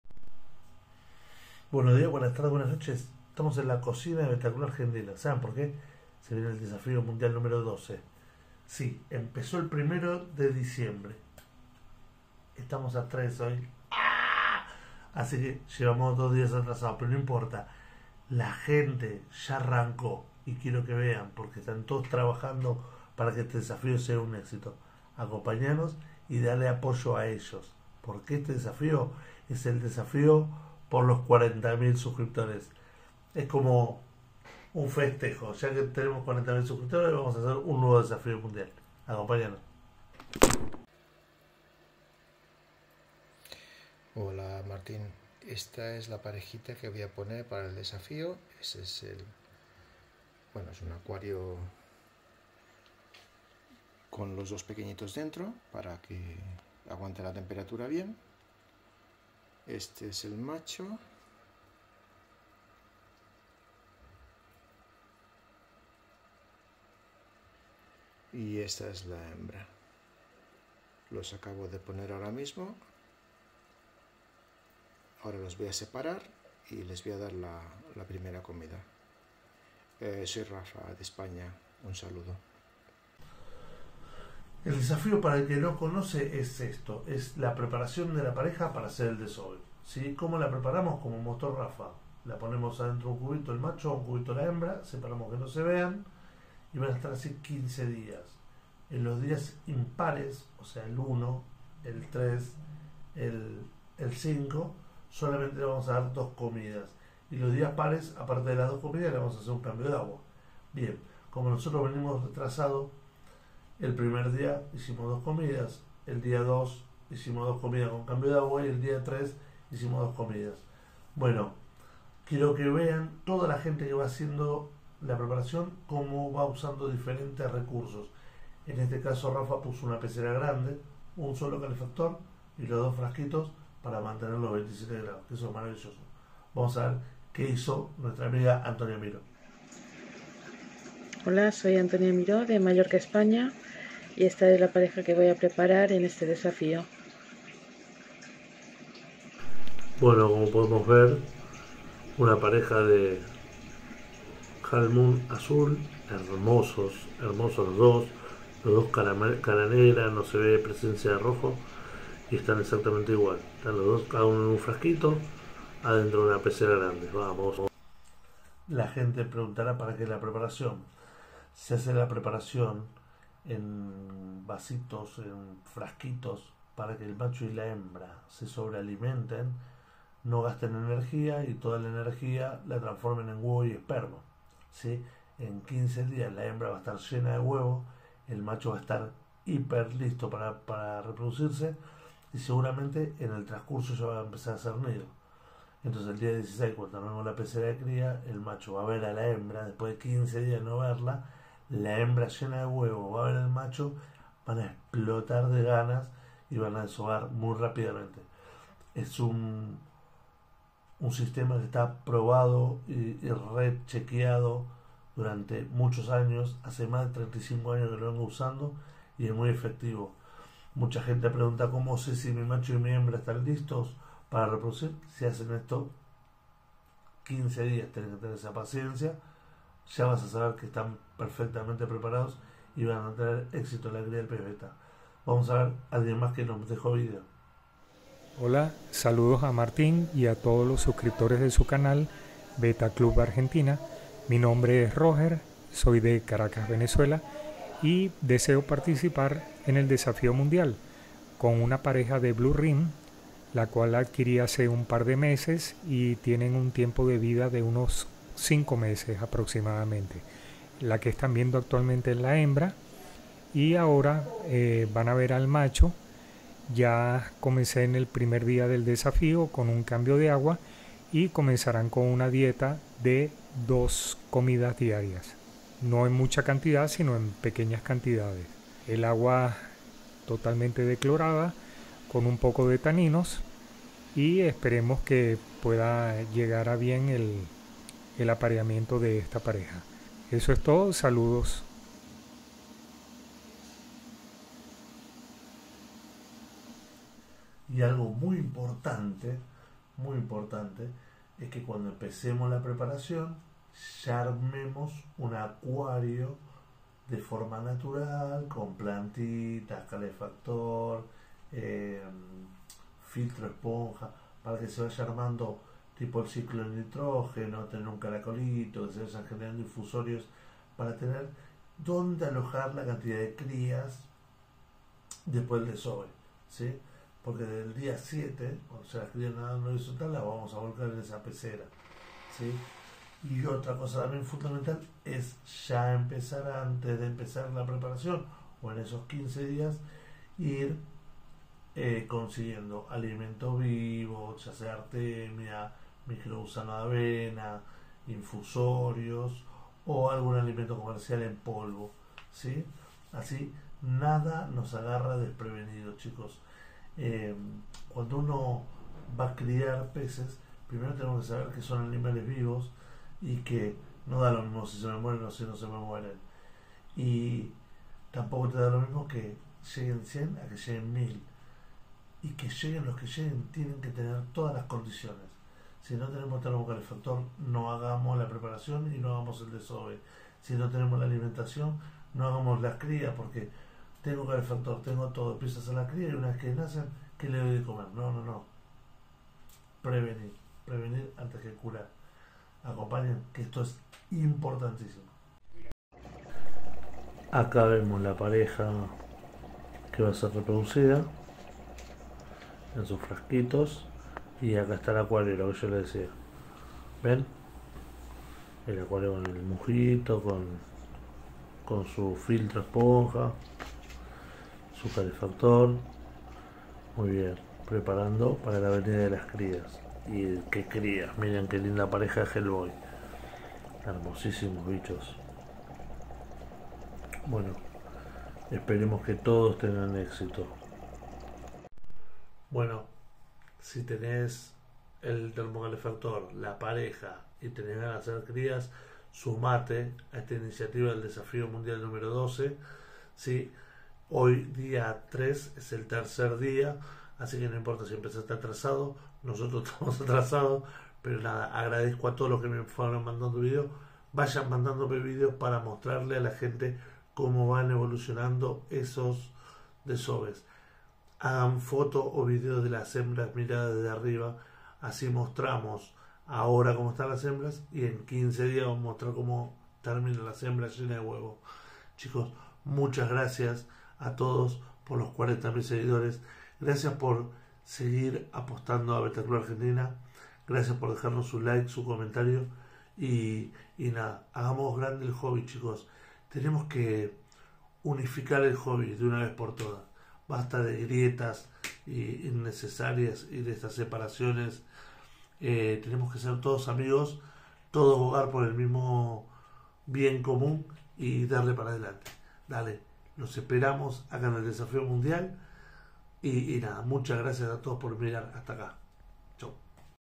Desafío mundial Bettaclubargentina nro12
con motivo de haber conseguido los 40000 suscriptores haremos un desove masivo en el mundo del pez betta aca en bettaclubargentina haremos una preparacion con los suscriptores del mundo bettero y vamos intentar tener desoves simultaneos